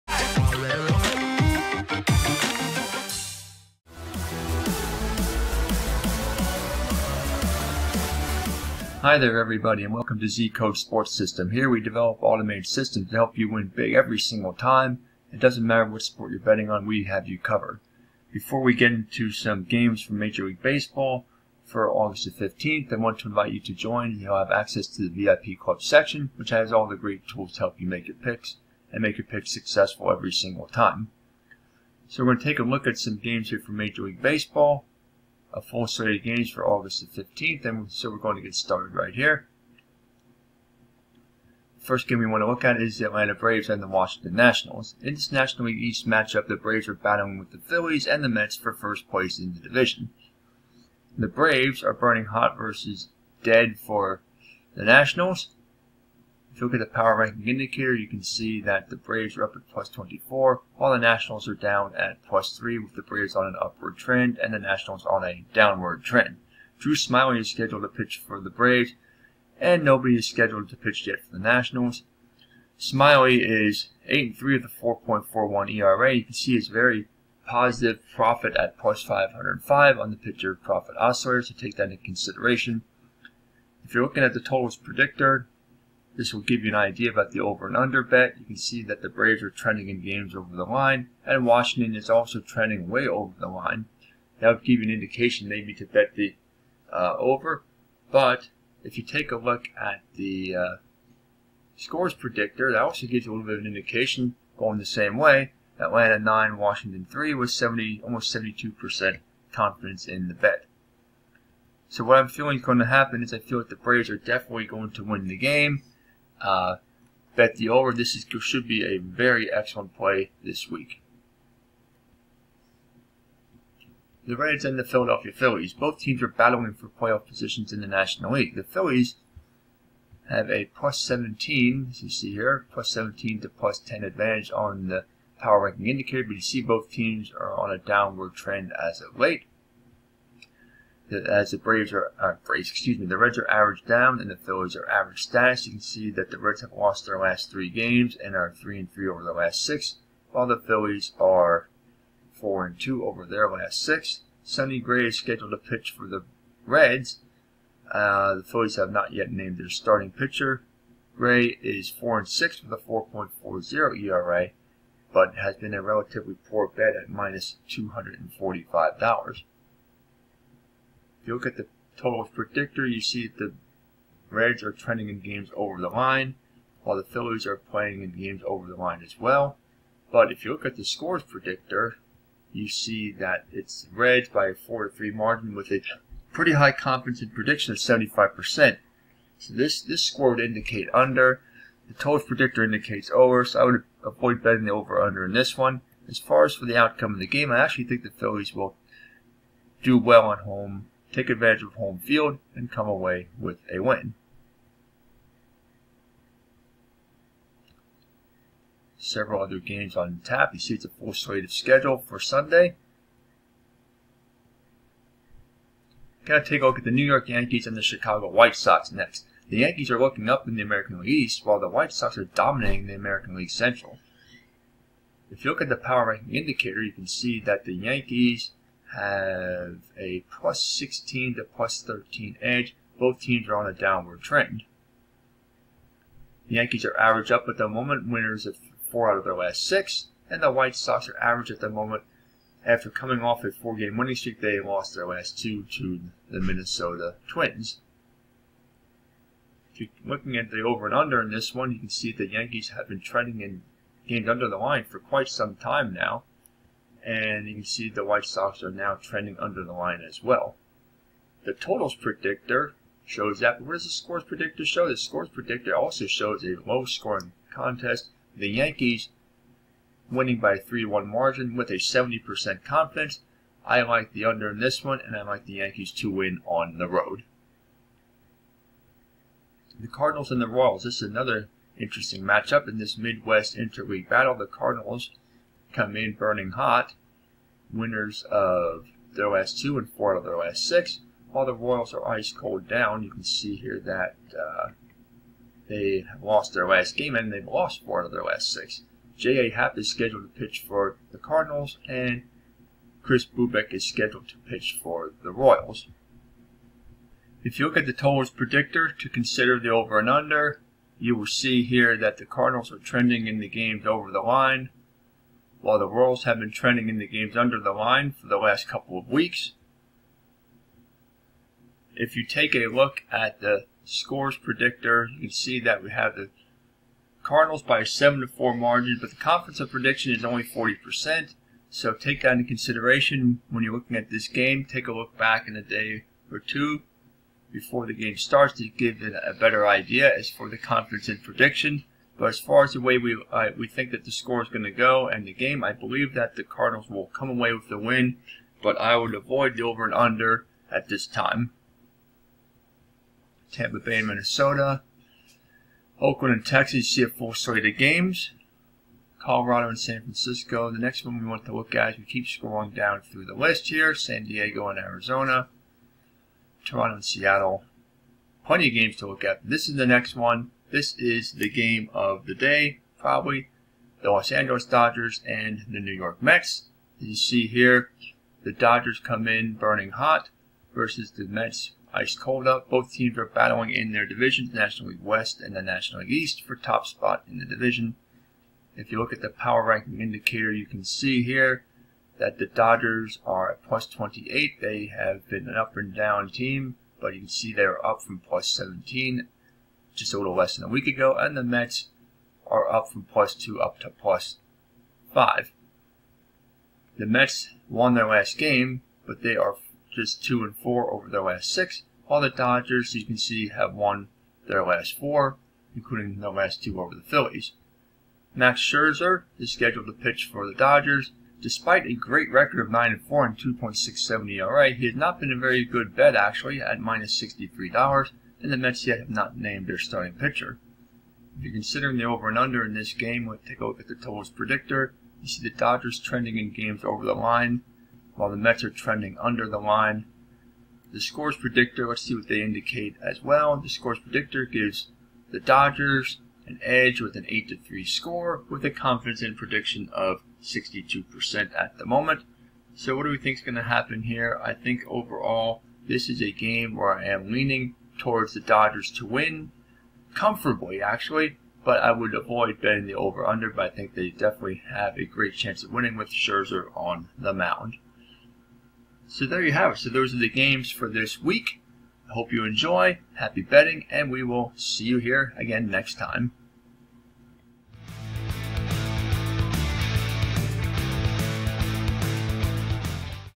MLB Picks August 15: Dodgers & Mets Battle in Possible Playoff Preview
With nearly three-quarters of the MLB season in the books, the divisional and league playoff races are heating up. There is a full slate of games for August 15 with the Dodgers and Mets headlining the action.

00:00 Lets Start!
00:44 Atlanta Braves versus Washington Nationals
02:10 Cincinnati Reds versus Philadelphia Phillies
03:39 New York Yankees versus Chicago White Sox
04:40 St. Louis Cardinals versus Kansas City Royals
06:10 Los Angeles Dodgers versus New York Mets